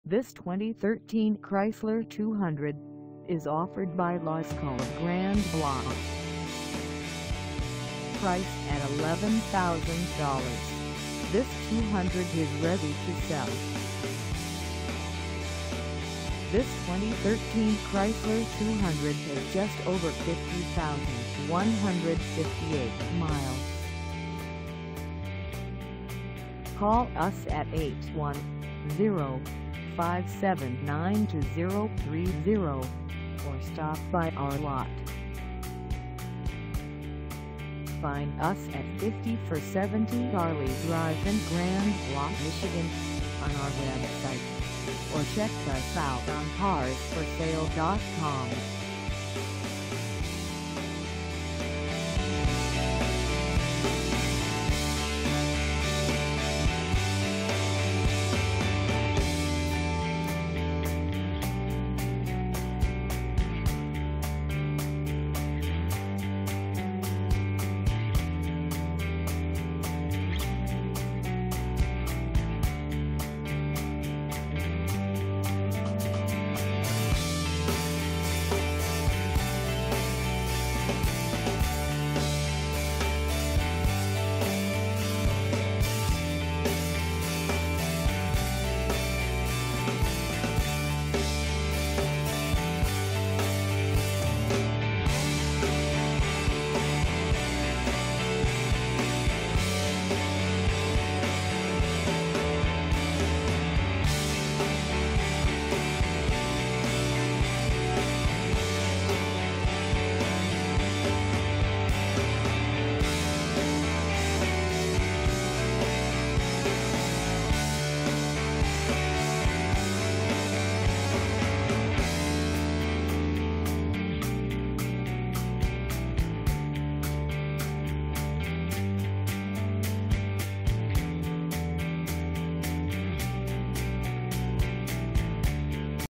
2013 Chrysler 200 Limited 4dr Sedan for sale in Grand Blanc,
This 2013 Chrysler 200 Limited 4dr Sedan is for sale in Grand Blanc, MI 48439 at Lasco of Grand Blanc.

Engine: 3.6L V6
Transmission: Automatic 6-Speed
Color: Black
Mileage: 50158
Contact Lasco of Grand Blanc 5470 Ali Dr Grand Blanc, MI 48439 to test drive this 2013 Chrysler 200 Limited 4dr Sedan today.